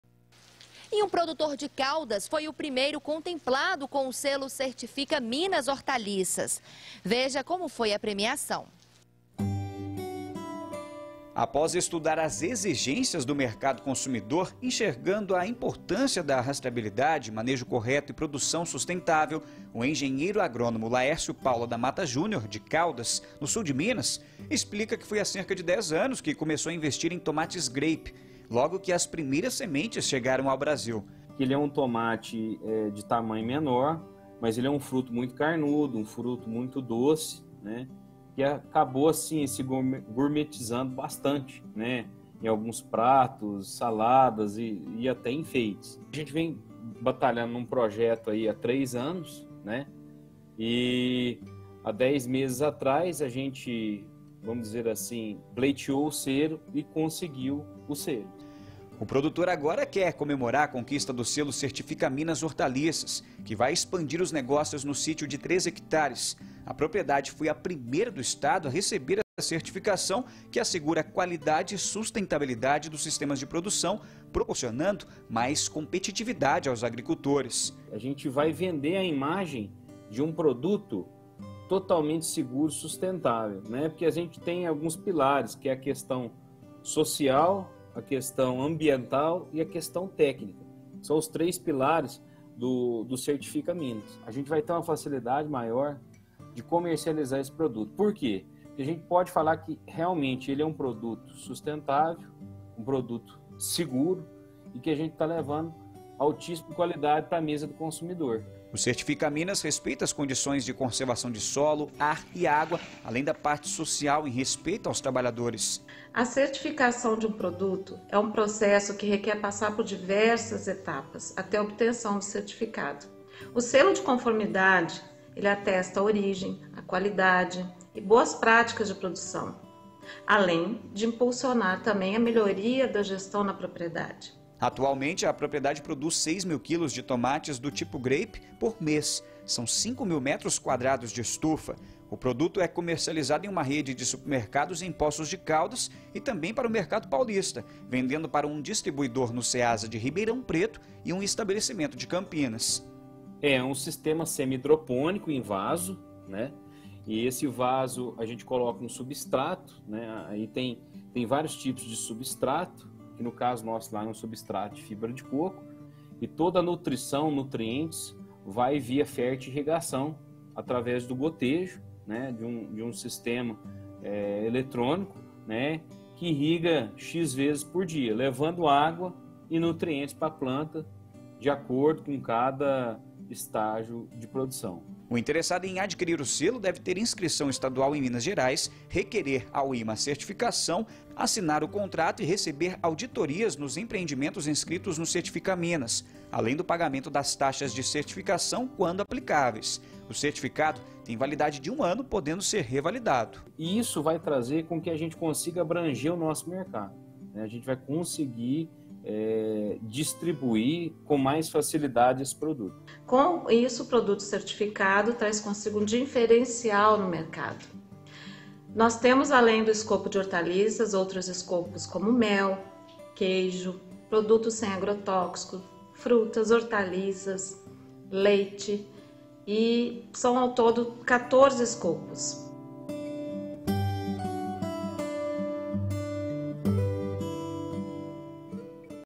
Produtor de Caldas é premiado
QUALIDADE E SUSTENTABILIDADE: PRODUTOR É PREMIADO | Um produtor de hortaliças de Caldas, no Sul de Minas, foi o primeiro premiado com o selo de qualidade no estado.